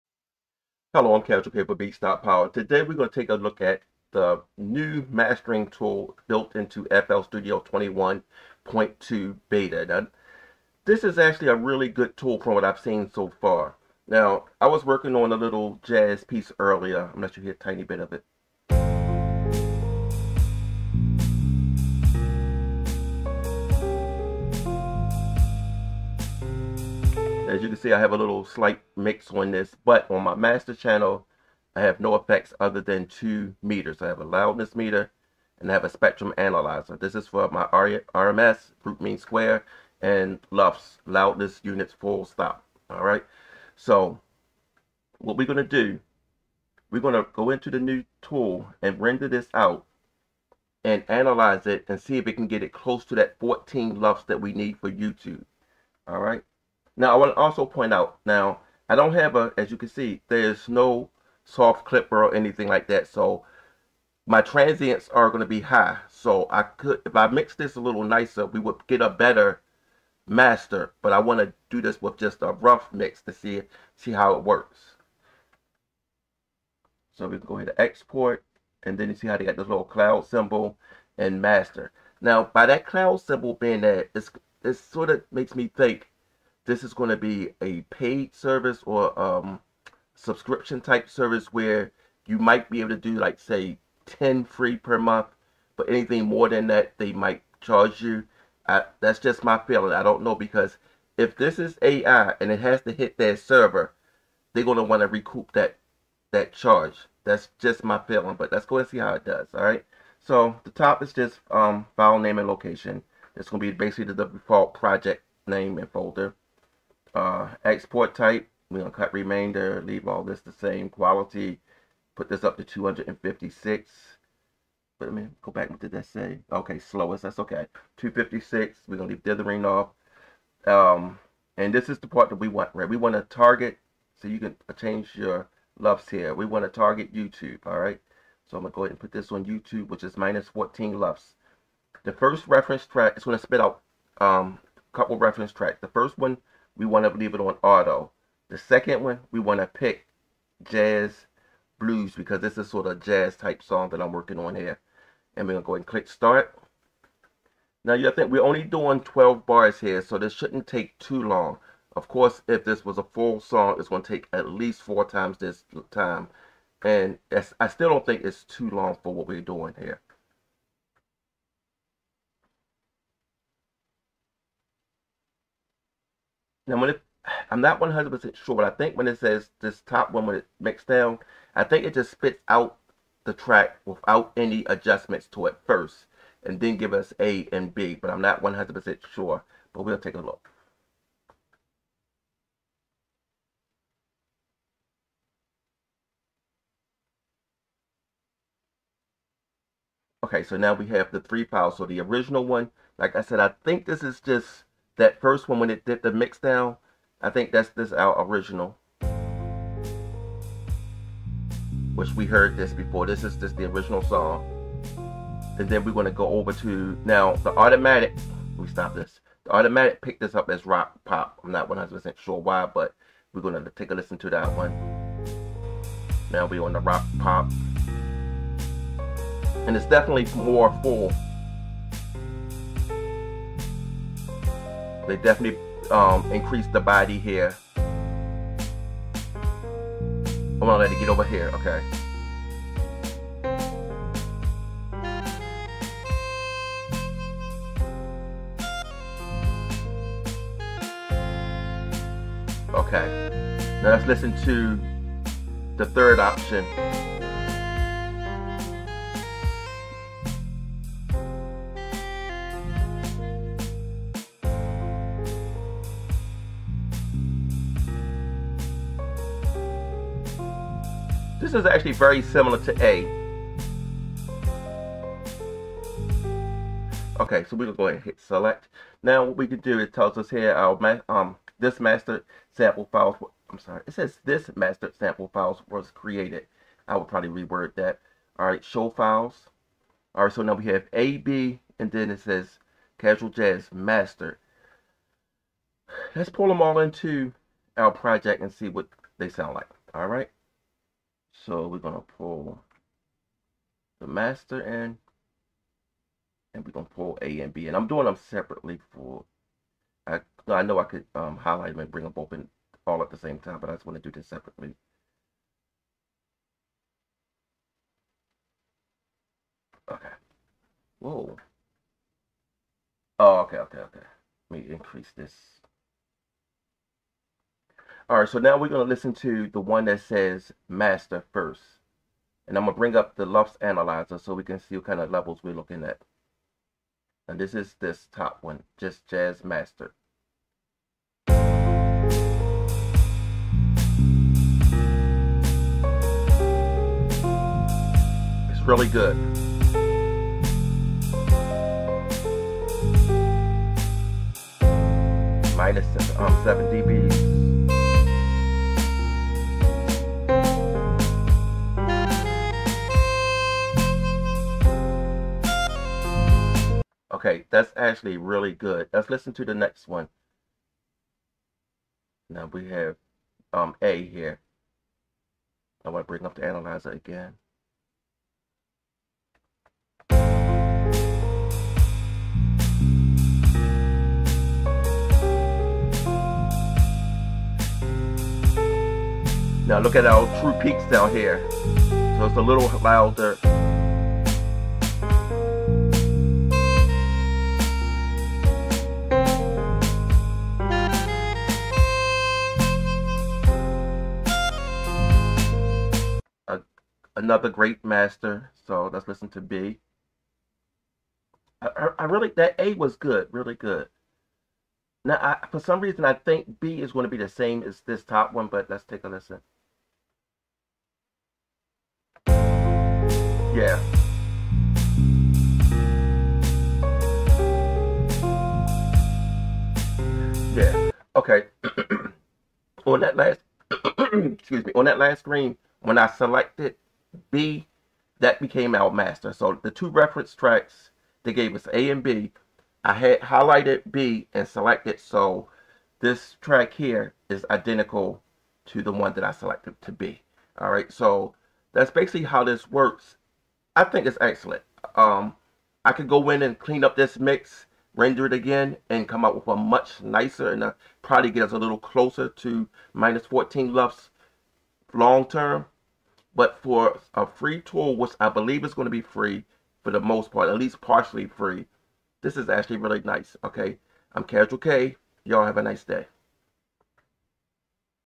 FL Studio 21.2 Beta AI Mastering
FL Studio 21.2 Beta AI Mastering. Another great addition to FL Studio. AI Mastering built into FL Studio is a game-changer.